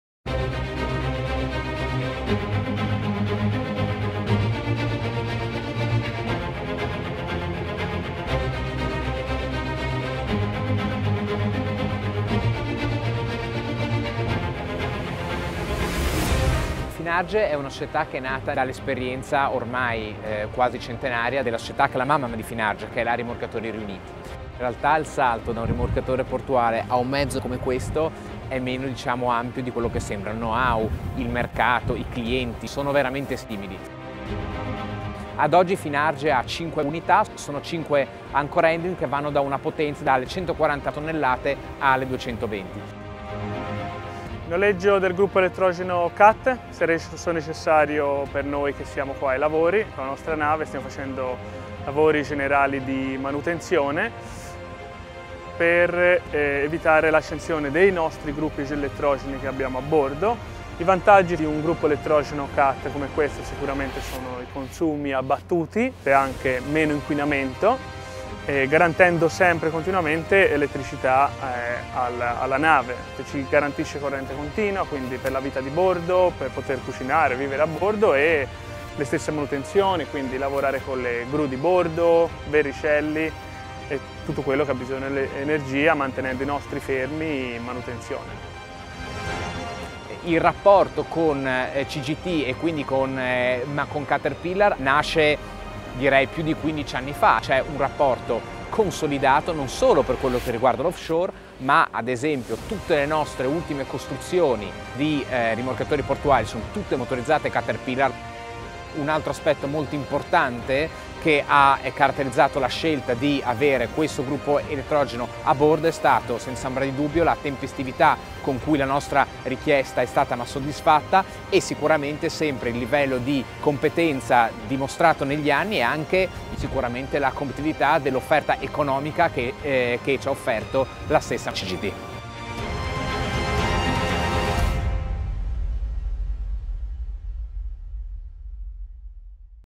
Energia a noleggio su un rimorchiatore
L'Energia del Rental Power CGT arriva davvero ovunque! Anche su un rimorchiatore attraccato nel Porto di Genova per le consuete attività di manutenzione.